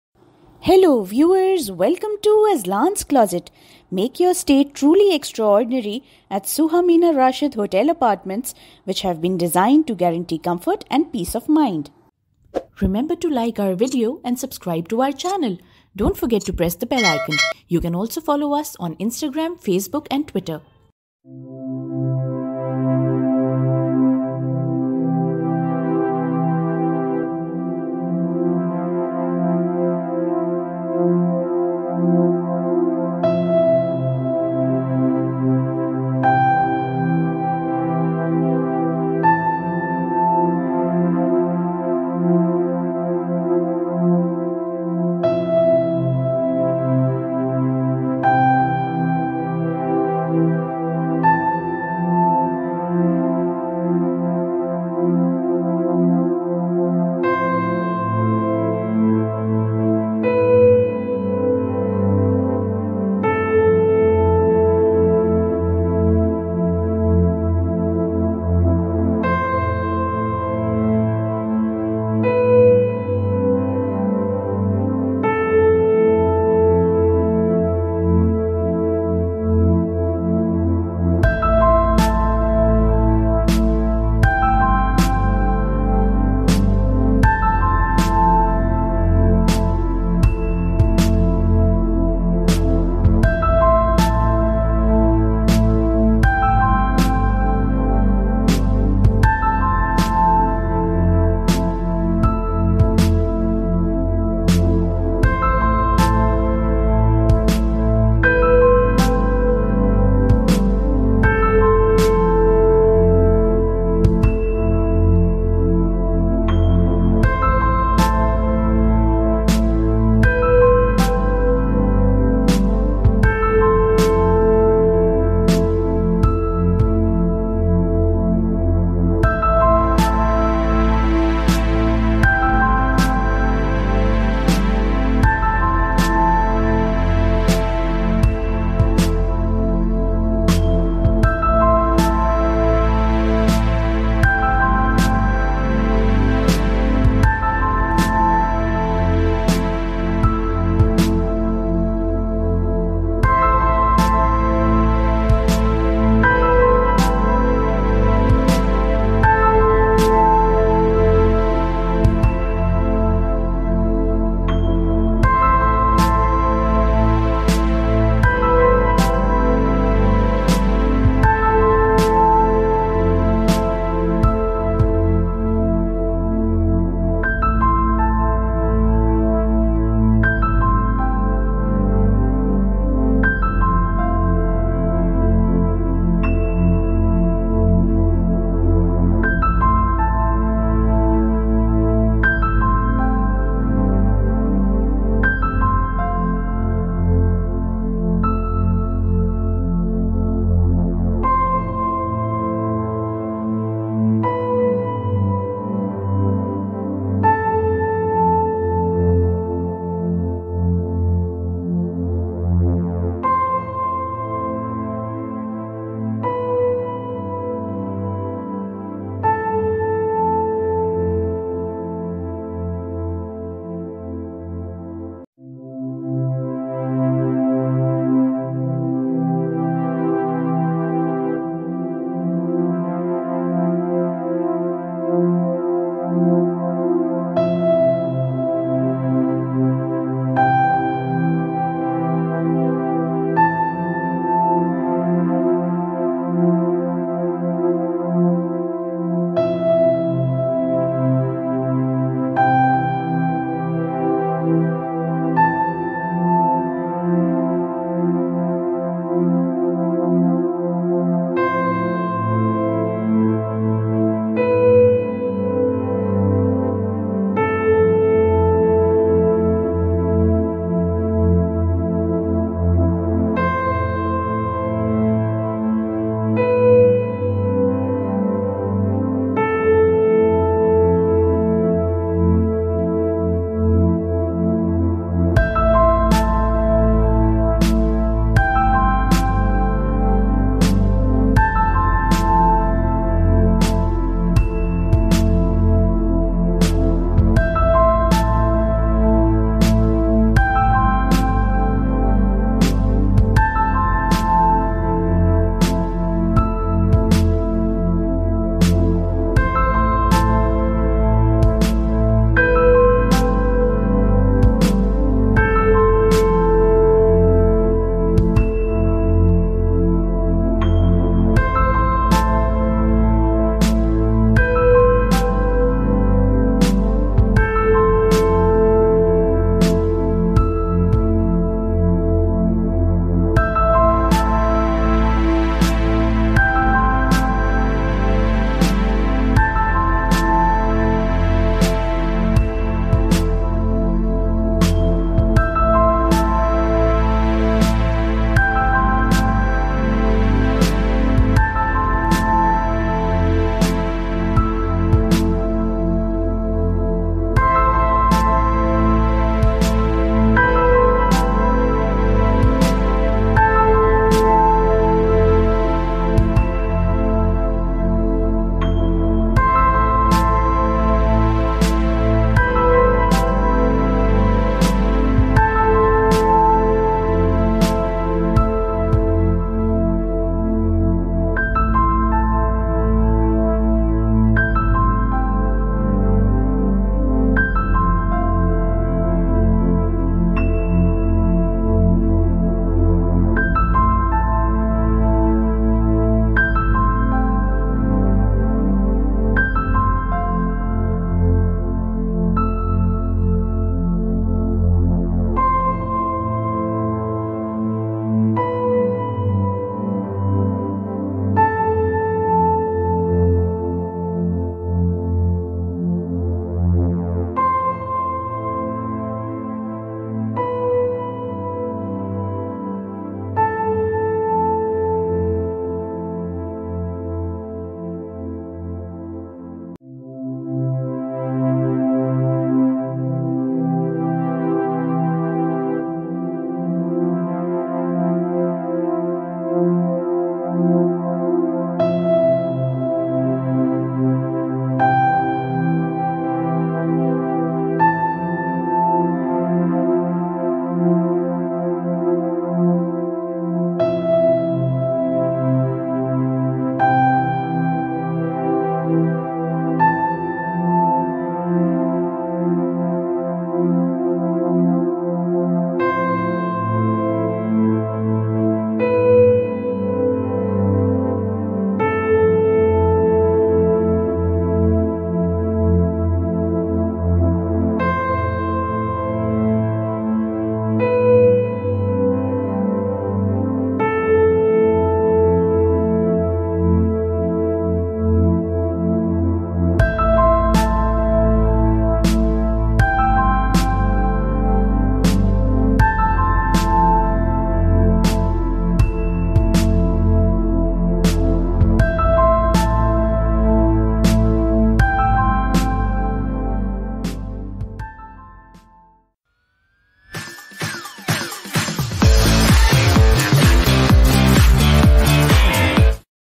SUHA Mina Rashid Hotel Apartment | 4 Star Hotel Dubai | Things to do in Dubai | Dubai Travel Vlogs
The amazing 4-Star SUHA Mina Rashid Hotel Apartments is centrally located in Bur Dubai.